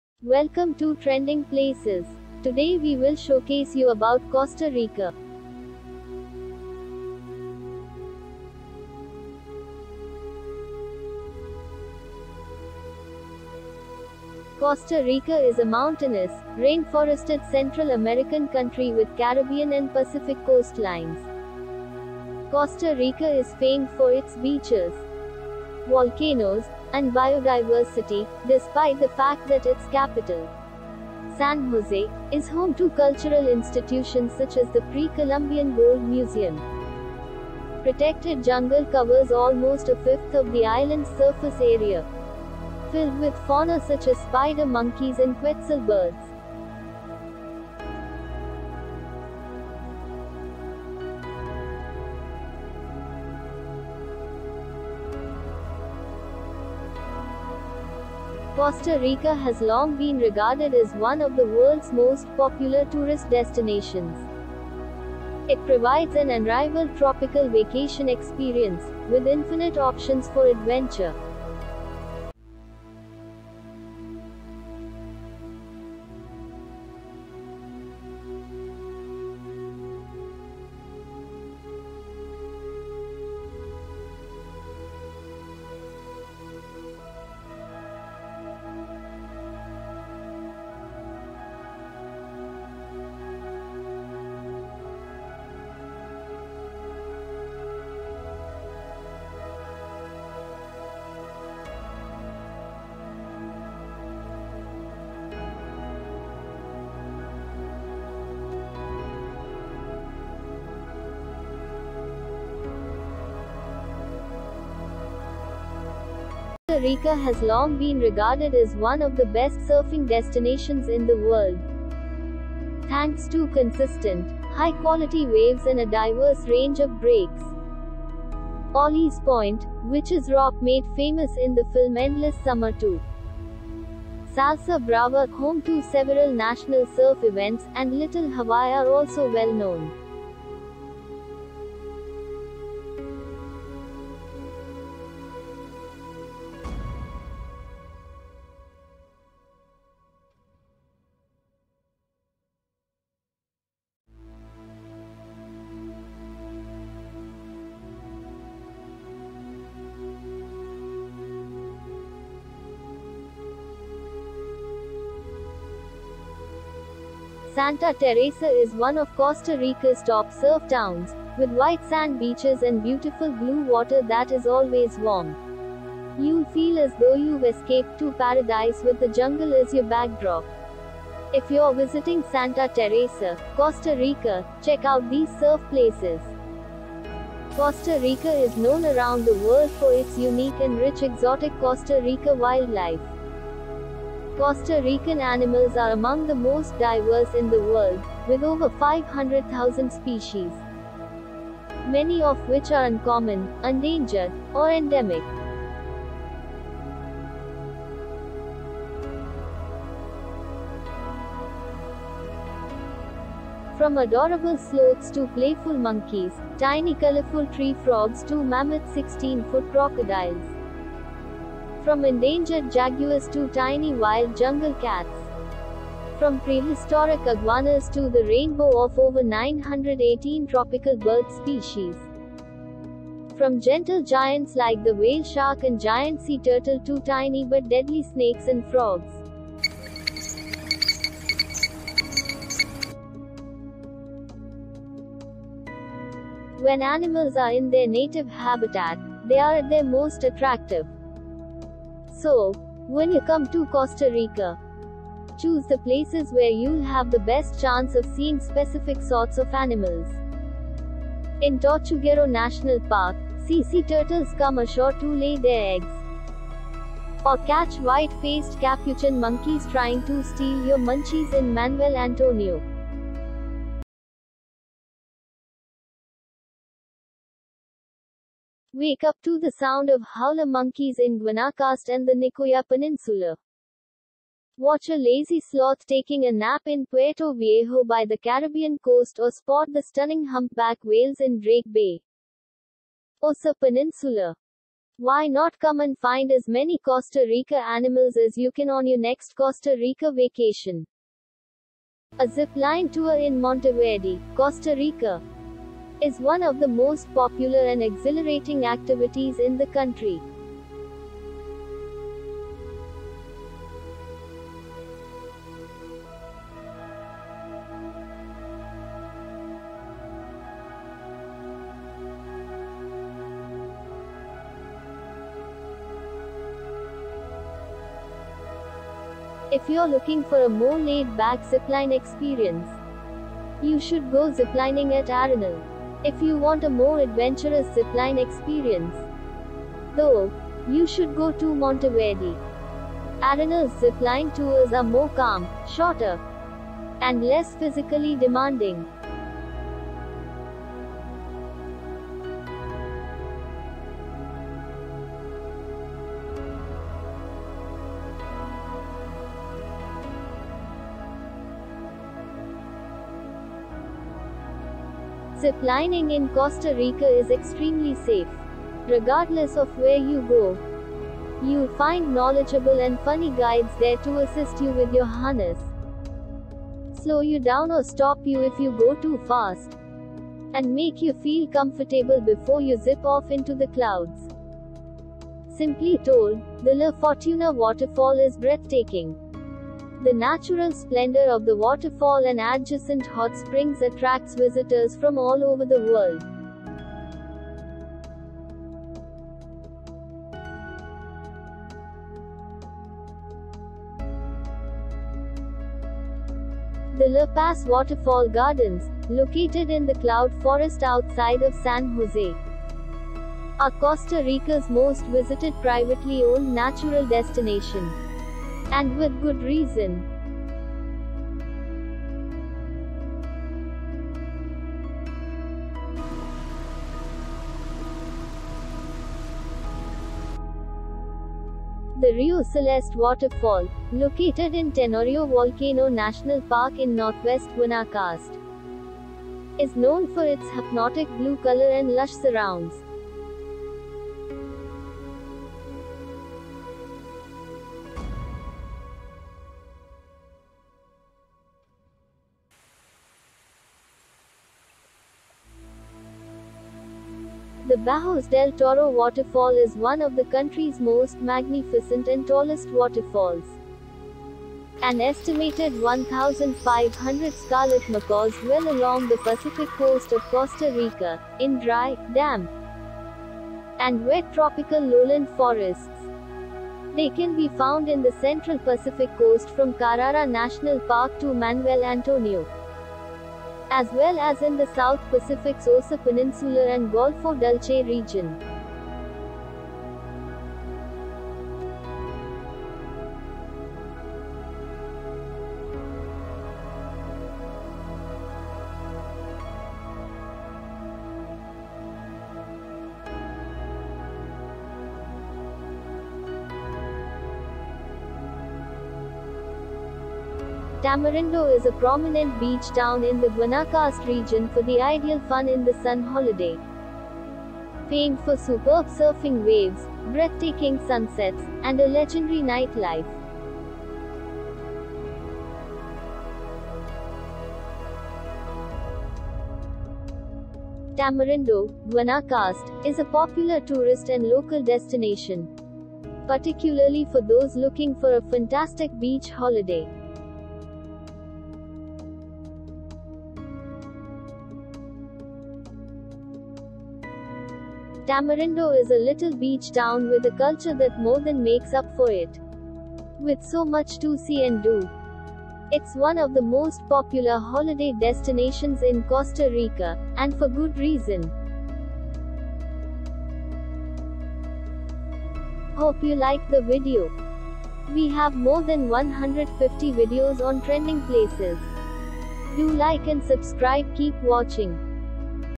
कोस्टा रिका, बेजोड़ उष्णकटिबंधीय अवकाश, Costa Rica, Unrivalled Tropical Vacation Experience | 2022
Costa Rica, Unrivalled Tropical Vacation Experience | Trending Places to see in 2022, कोस्टा रिका, बेजोड़ उष्णकटिबंधीय अवकाश

Costa Rica has long been one of the most popular tourist destinations in the world. With its golden beaches, abundant wildlife, and endless opportunities for adventure, it offers an unparalleled tropical vacation experience.
Costa Rica is a rugged, rainforested Central American country with coastlines in the Caribbean and Pacific. Though its capital, San Jose, is home to cultural institutions like the Pre-Columbian Gold Museum, Costa Rica is known for its beaches, volcanoes, and biodiversity. Roughly a quarter of its area is made up of protected jungle, teeming with wildlife including spider monkeys and quetzal birds.